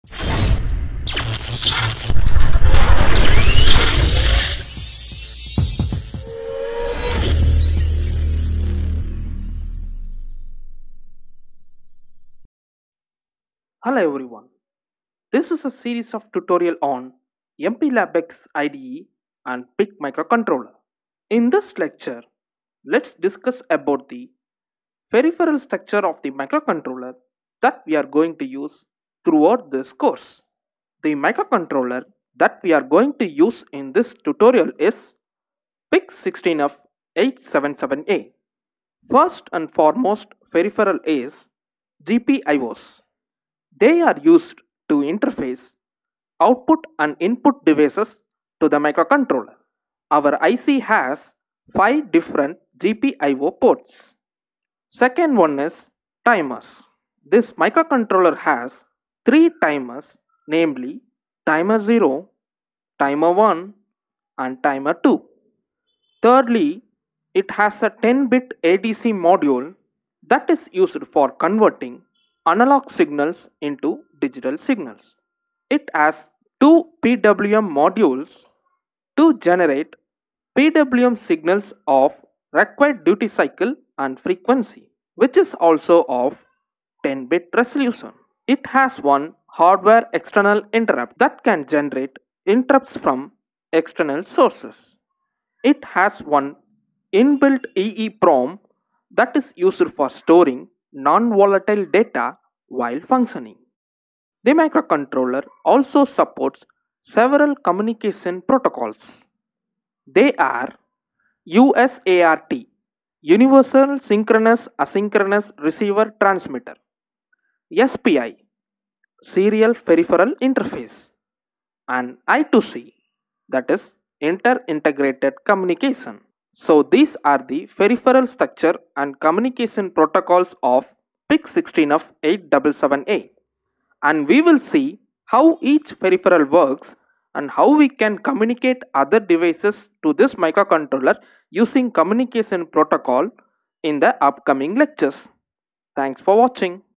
Peripheral Structure of PIC16F877A microcontroller
You will acquire knowledge on peripheral structure and communication protocols supported by the microcontroller PIC16F877A in this lecture ,This is a course on programming in MPLAB X IDE using XC8 compiler for the microcontroller PIC16F877A, For learning this course for free with all the documents and necessary source code for all the lectures